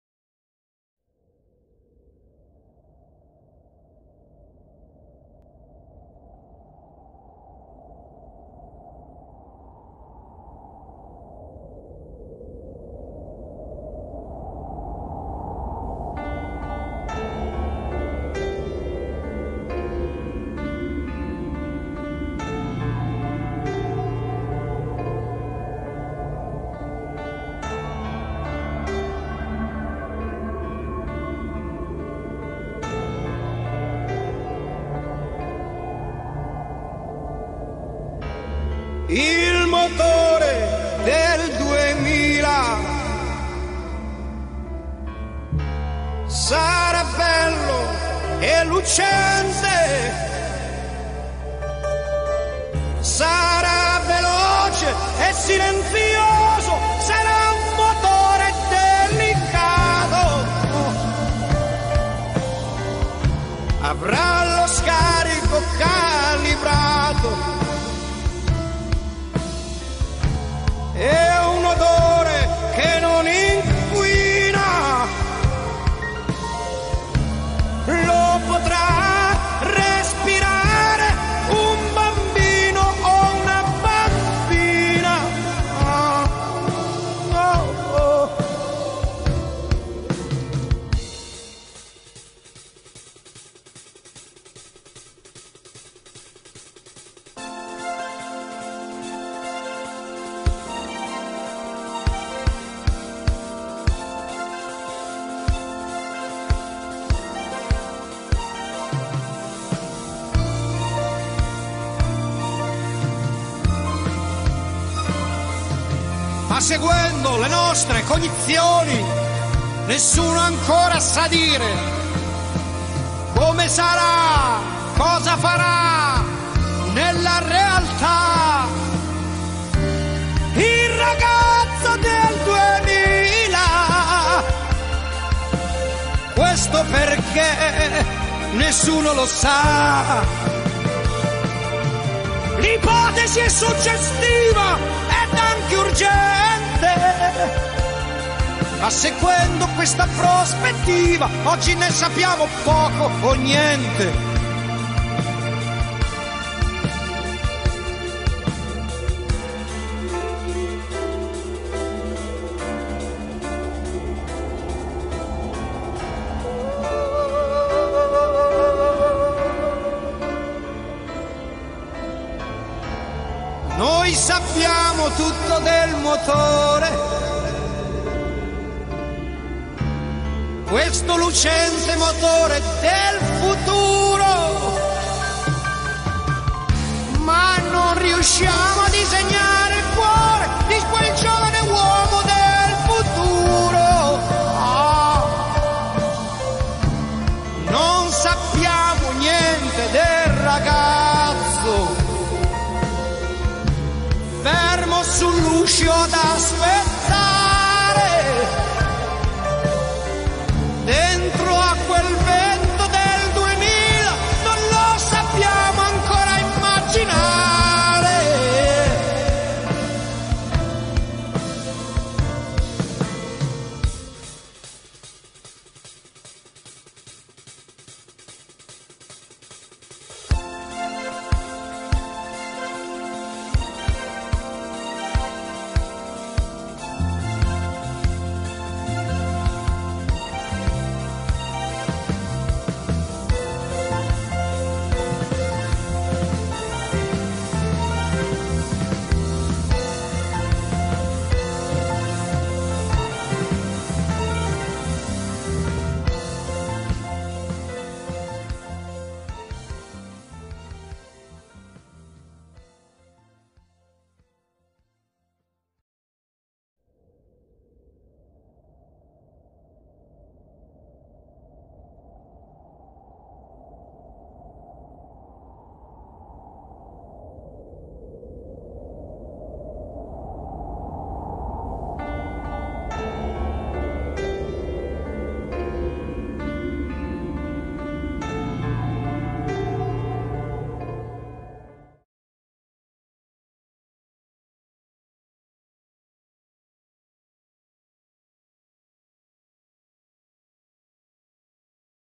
The best Italian Youngtimers
L'industria automobilistica italiana oggi ha sicuramente perso quel primato di eccellenza di cui ha goduto nel mondo fino agli anni 90. È giusto quindi ricordate ed apprezzare gli ultimi prodotti di notevole fattura. A mio avviso ritengo questi modelli di Lancia, Fiat e Alfa Romeo i migliori da collezionare come Youngtimer per numerosi motivi, che comunque cito nel filmato solo sommariamente.